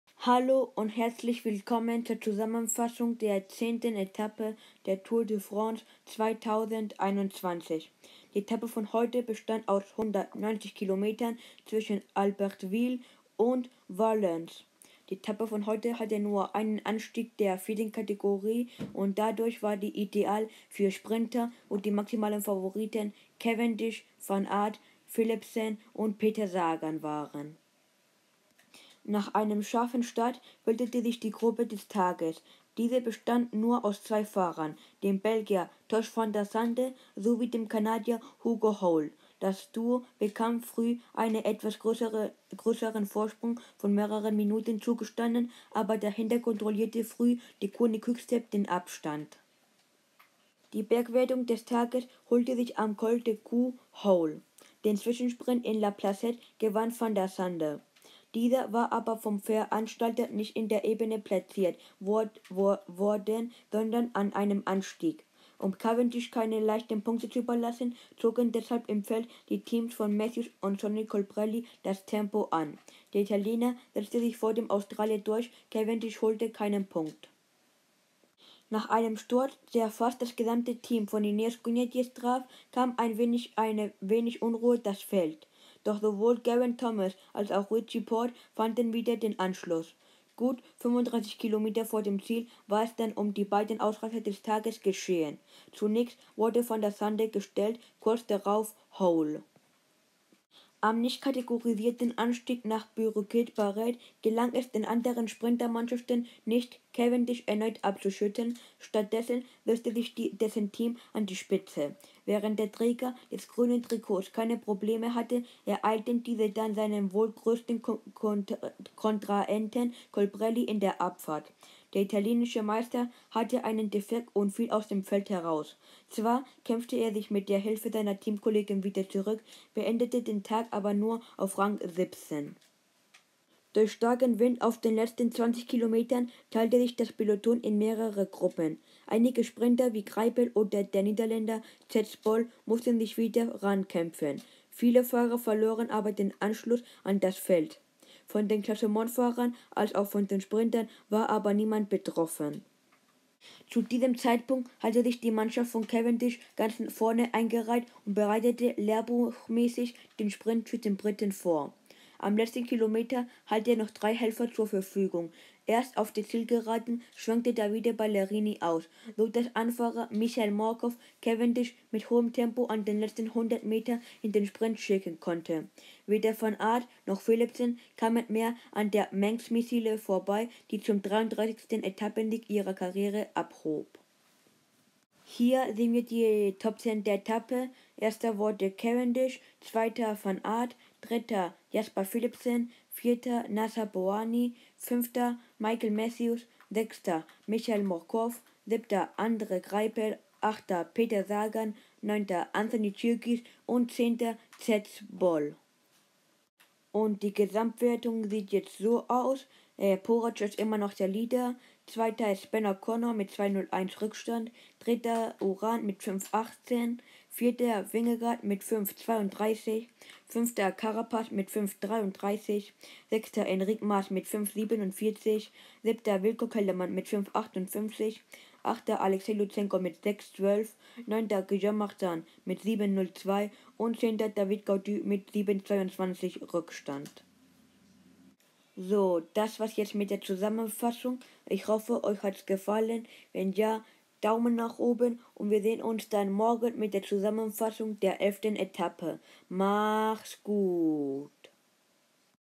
ZUSAMMENFASSUNG ETAPPE 10 ➤ TOUR DE FRANCE 2021 🇫🇷 Nur noch ein Sieg für den Rekord
Etappe 10 der Tour de France 2021  wurde ausgetragen. Der Sieg war für Mark Cavendish im Endspurt mit einem perfekten Start seines Teams. Damit liegt er einen Einzelsieg hinter Eddy Merckx' Rekord von 34 Siegen auf der Tour. Favoriten wie Tadej Pogacar, Richard Carapaz, Rigoberto Urán oder Enric Mas kamen ohne Probleme an. Morgen werden wir den Mont Ventoux besteigen, wo wir auch einen Kampf um die Klassifizierung des Berges unter der Leitung von Nairo Quintana sehen werden. Ich überlasse Ihnen die komplette Gesamtwertung der Tour 2021. Alles in den Highlights der Dany Pro Cycling Zusammenfassung dieses internationalen Radsportwettbewerbs.